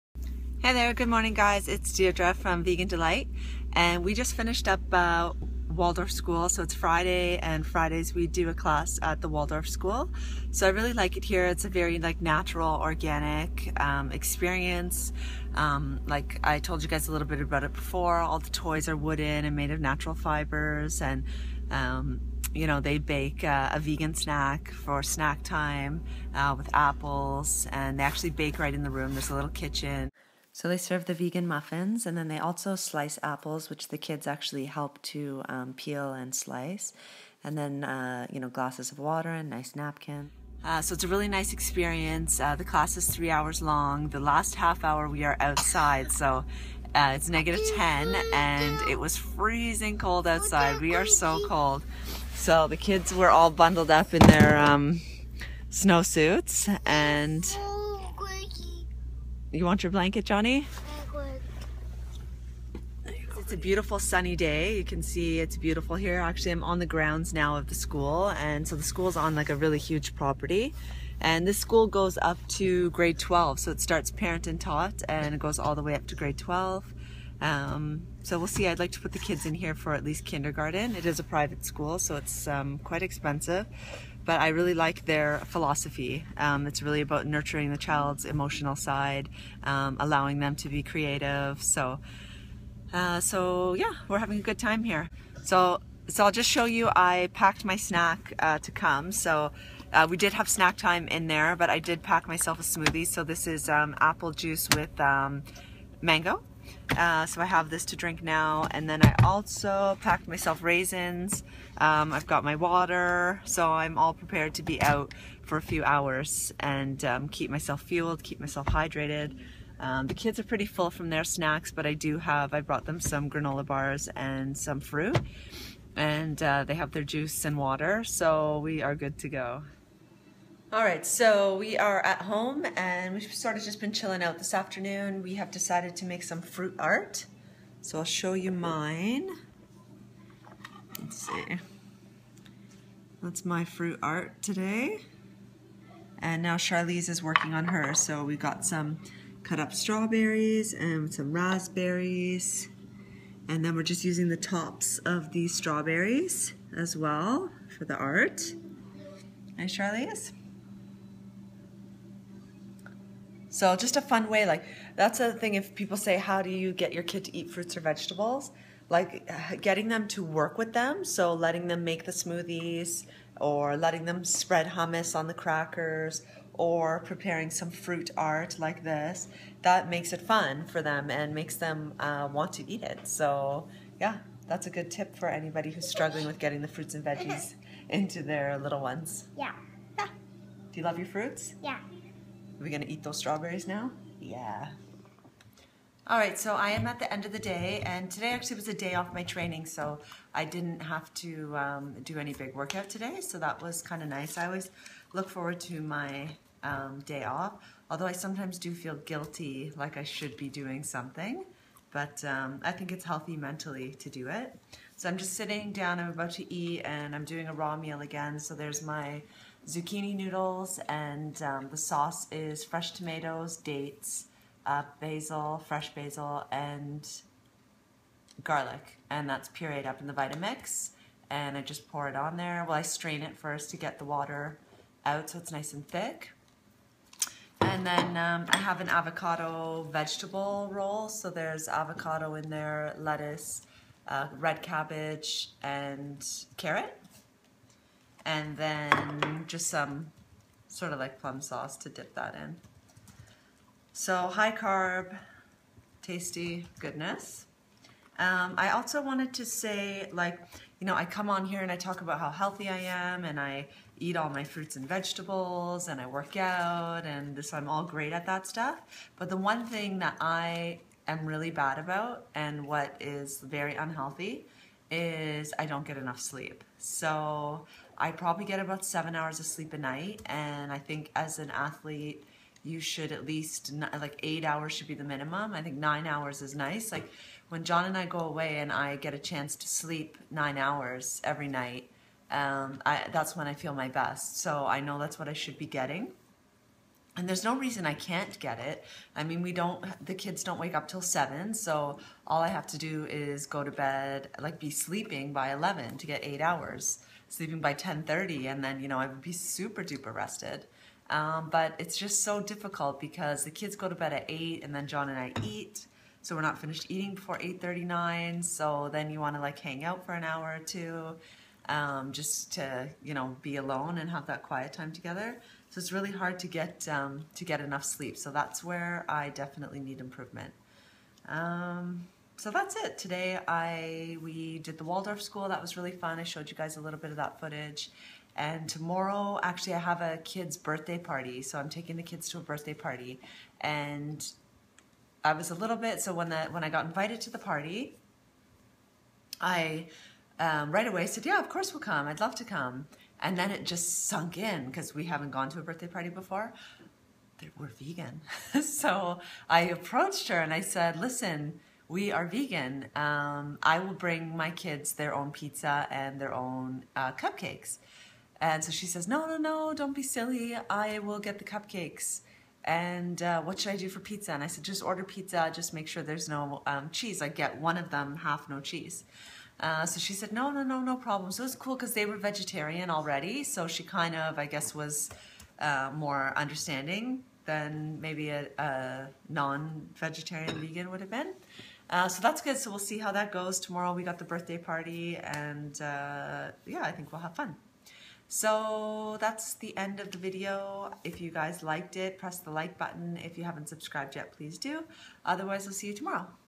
I tell you my UNHEALTHY habit + we are at Waldorf today!
We start the day out at the Waldorf school. Charlize and I make fruit art in the afternoon. Then I tell you what my unhealthy habit is.